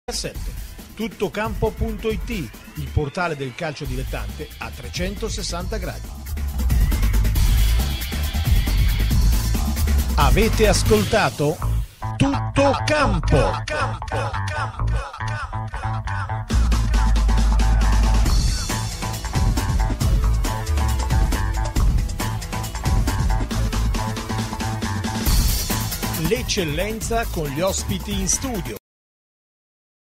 L'Eccellenza di Tuttocampo.it (part 4 di 4)
I campionati di Eccellenza in Italia raccontati dagli inviati di Tuttocampo.it